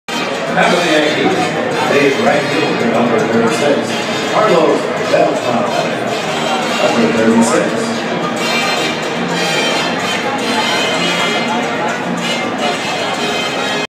Carlos Beltran walk up song
Carlos Beltran walk up song 2015
Toronto Blue Jays @ New York Yankees 9/13/15 Yankees win 5-0